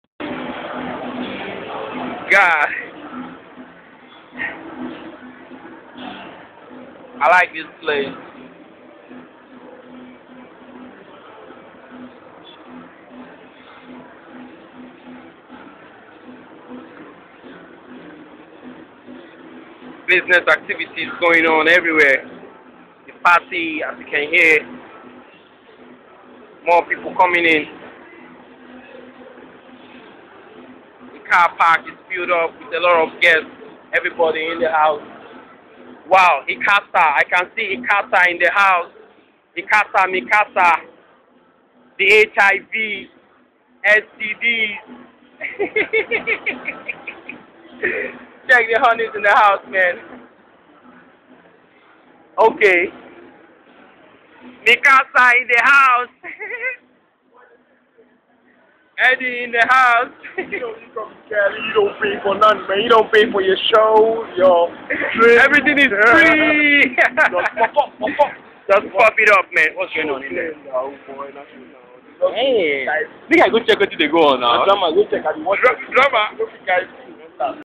Calabar Fiesta 2008
Calabar festival just begun, life action from Cultural Centre.